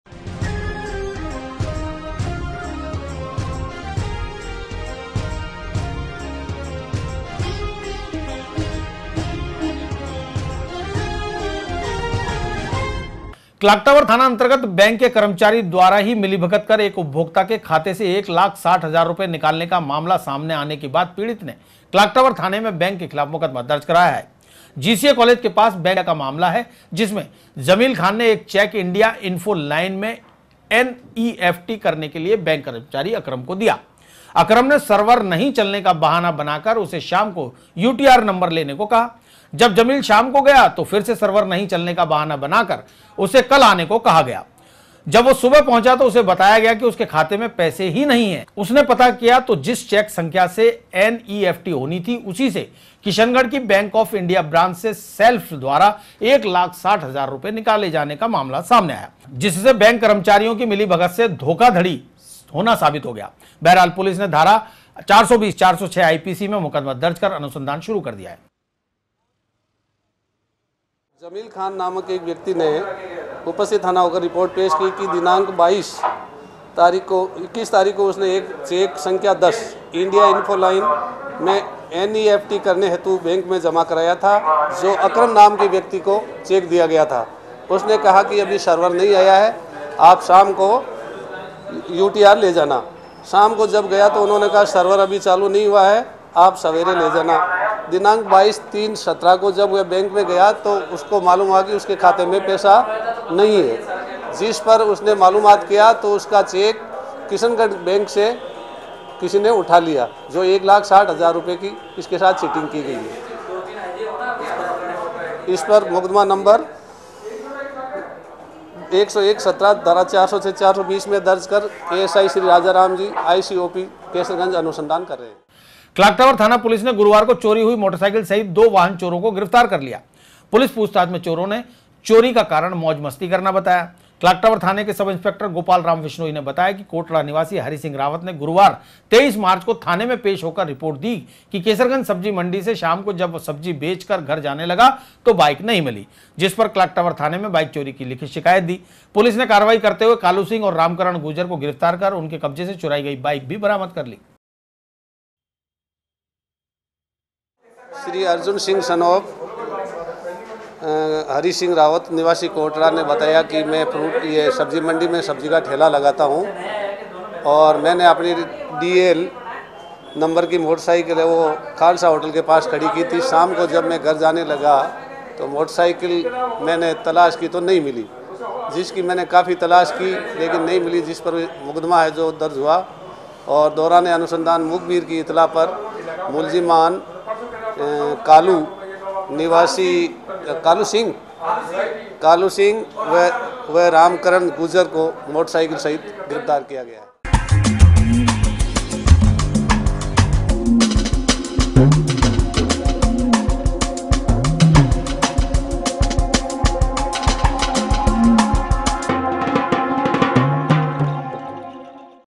बैंक के कर्मचारी ने की धोखाधड़ी। 2 चोर को बाइक सहित किया गिरफ्तार।
जीसीए के नजदीक बैंक के कर्मचारी ने ग्राहक के चैक से की धोखाधड़ी। 1 लाख 60 हजार रूपये किये विड्रोल। क्लॉक टॉवर थाने में मुकदमा दर्ज। वहीं क्लॉक टॉवर थाना पुलिस ने 2 चोर को बाइक सहित पकड़ा। 24 March 2017